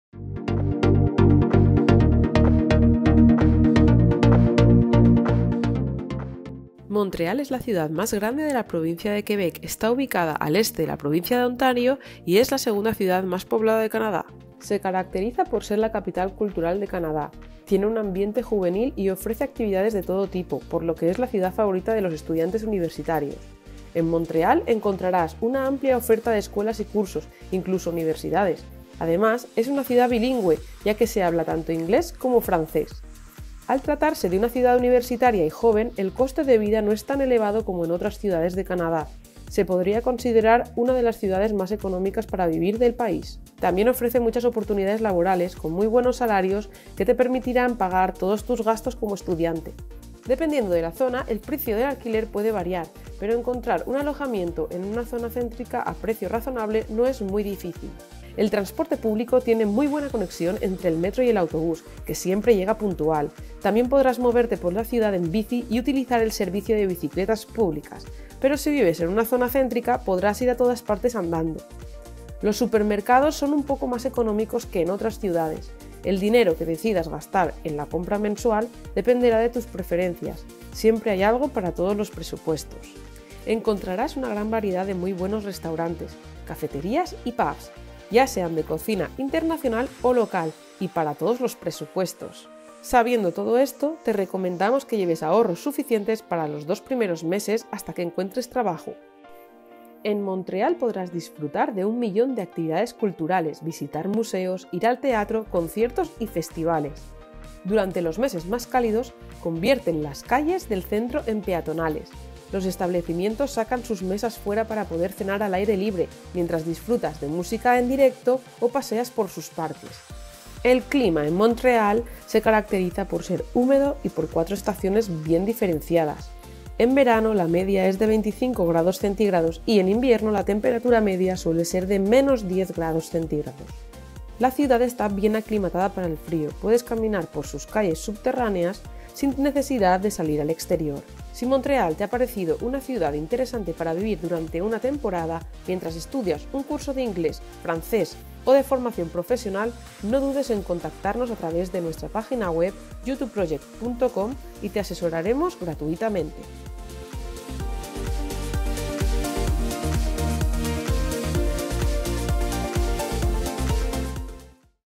Estudiar en Montreal 🎡 🇨🇦 Gastos, transporte, alojamiento, ocio, clima ¡y más! ✅
¿Quieres estudiar en Montreal? 📚 🇨🇦 Esta preciosa ciudad canadiense es el destino de una gran cantidad de estudiantes universitarios y estudiantes de idiomas. Es la más grande del estado de Quebec y, además, es una ciudad bilingüe por lo que podrás estudiar inglés y/o francés, entre otros tipos de cursos.

¿Quieres unirte a nuestros estudiantes en Montreal?  ¡EscríbenOOs! Nosotras te ayudamos 😊👉🏽